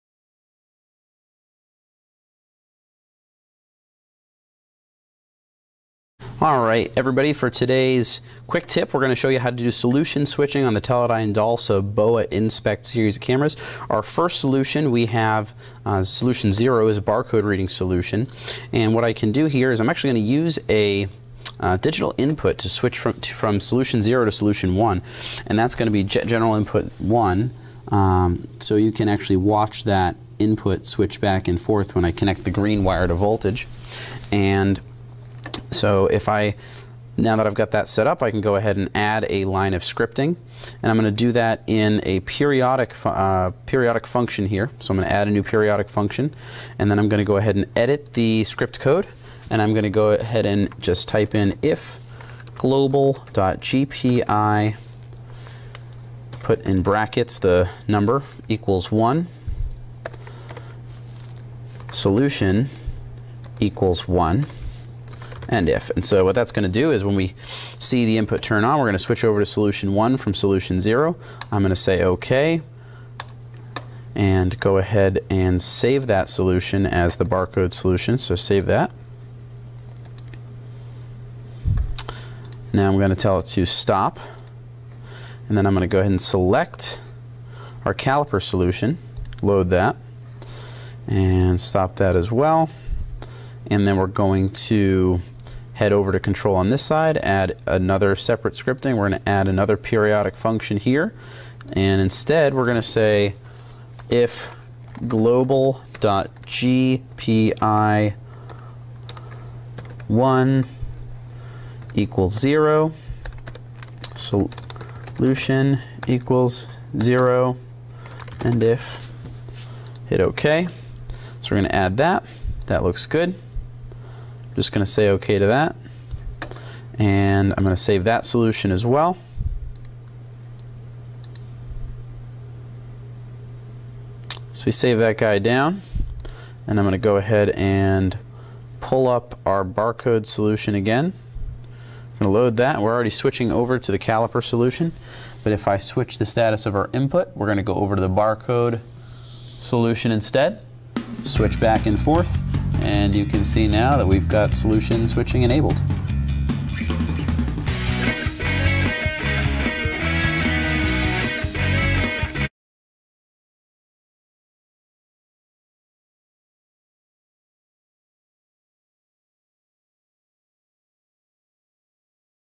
Teledyne Dalsa Machine Vision Solution Switch
Learn how to configure our Teledyne Dalsa BOA Smart Camera for Solution Switching running iNspect software.  Solution Switching is the ability for us to use a digital input to change our program on the fly.  This is useful when a machine changeover causes the elements of a vision inspection to change quickly and easily.  By simply triggering a digital input, we can have the smart camera intelligently switch between multiple pre-programmed inspection routines so that the right inspection is running.

Let us help with your manufacturing automation solution.